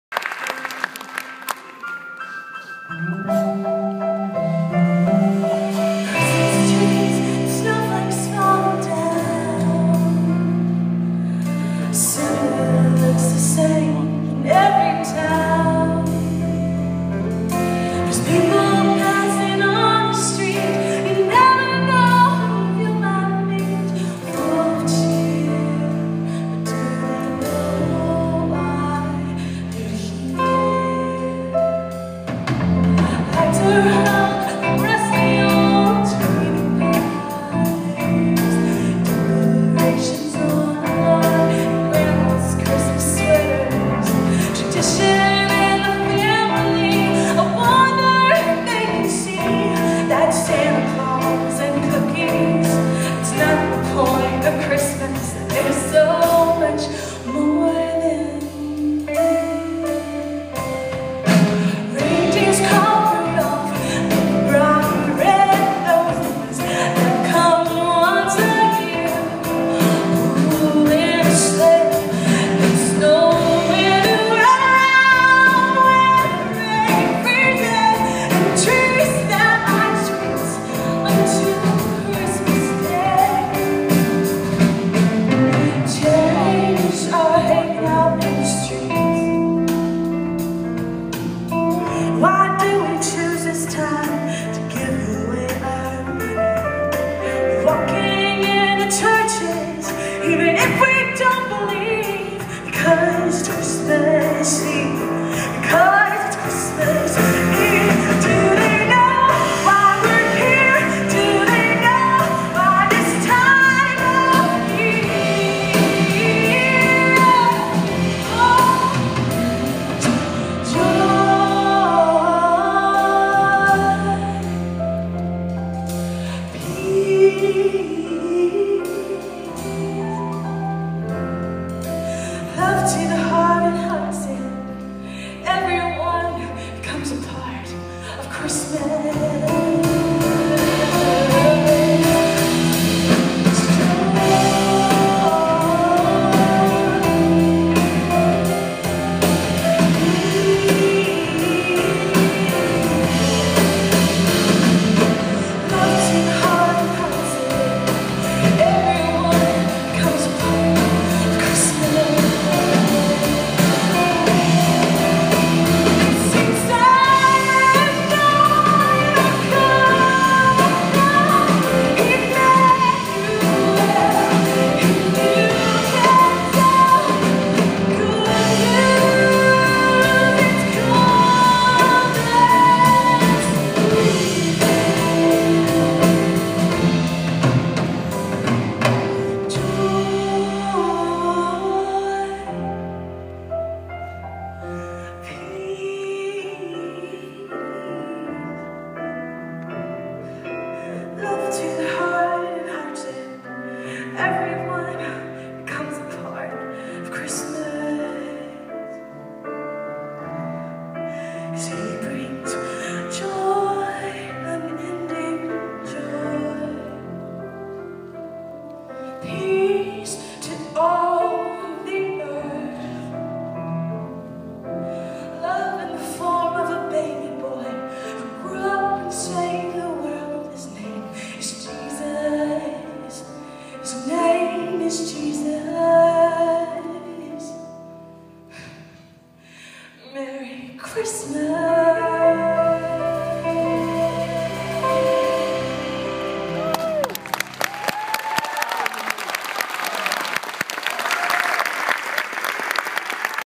Rock and Roll Christmas Show
Robert and Kevin performing with Holly Hartman.  Holly is a wonderful singer and songwriter.  This is one of her original songs.